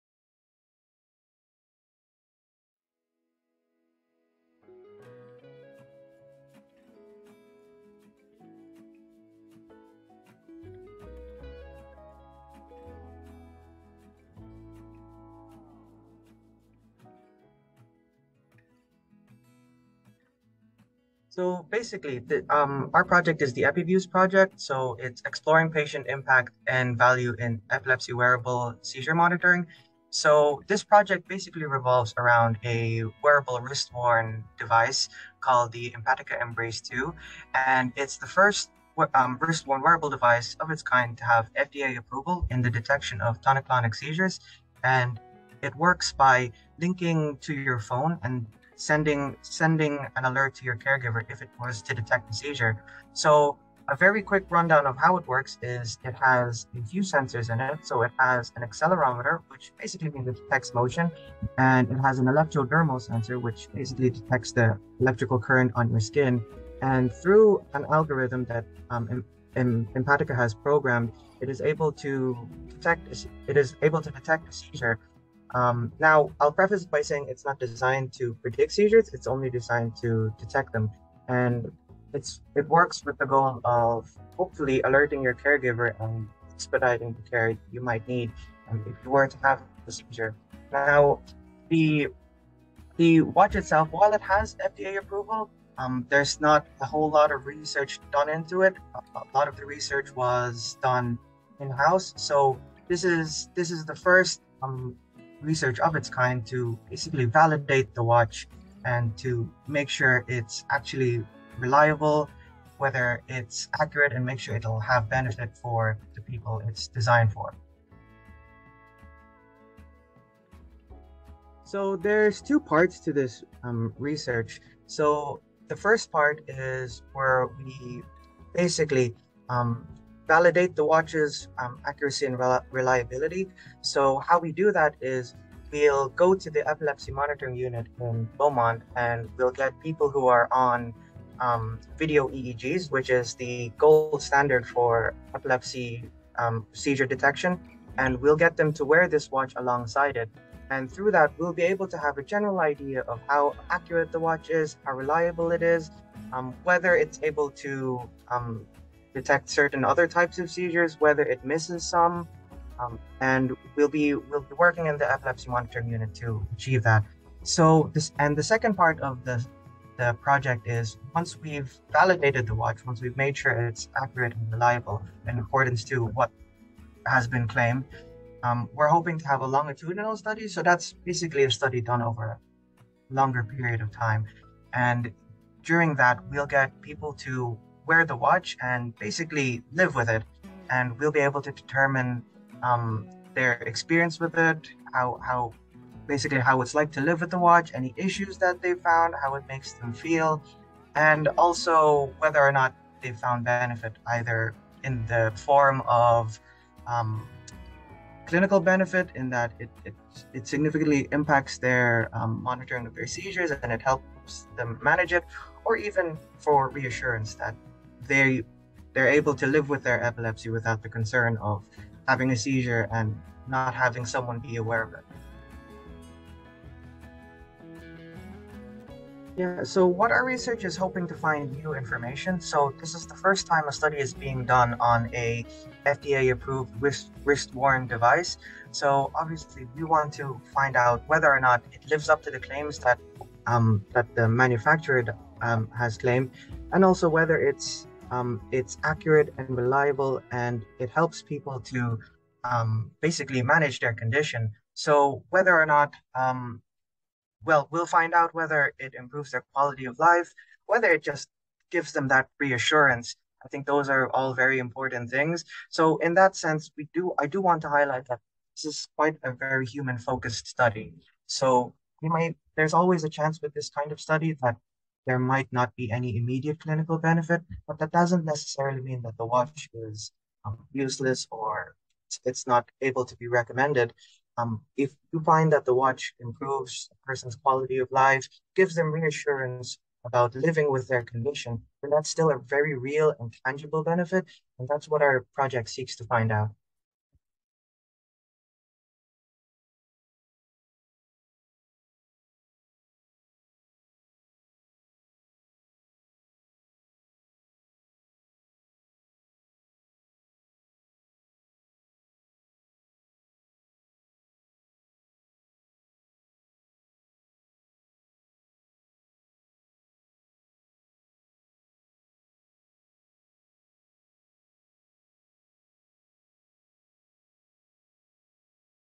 Epilepsy Ireland funds new study on the impact of wearable devices in epilepsy.
We speak to RCSI PHD Researcher John David Damalerio about his Epilepsy Ireland supported research investigating the value and impact of epilepsy wearables.